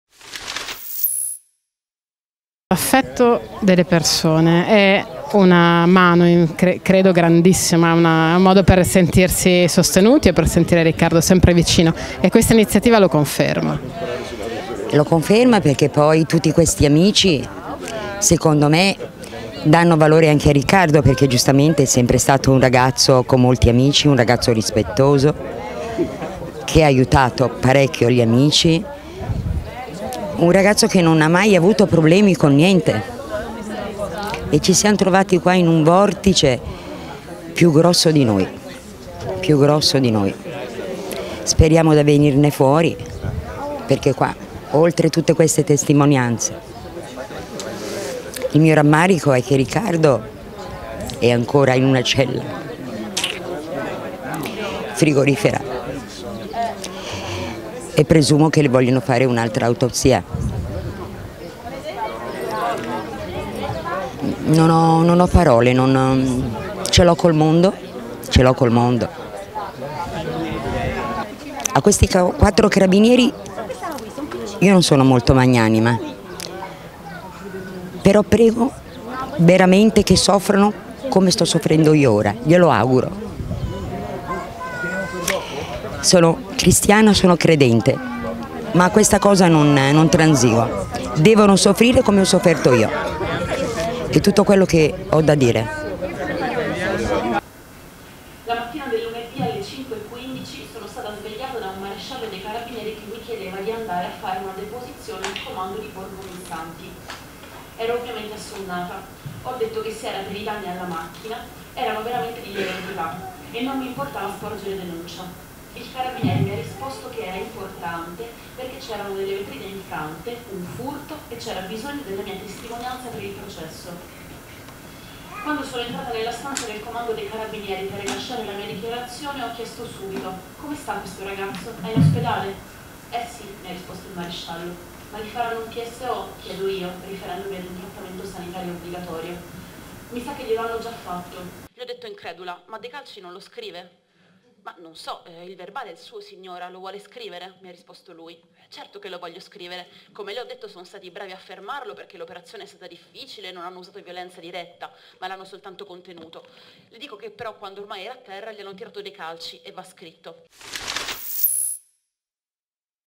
Firenze, testimoni del caso Magherini: "Non verbalizzate nostre dichiarazioni su calci su Riccardo"
di Max Brod e Silvia D'Onghia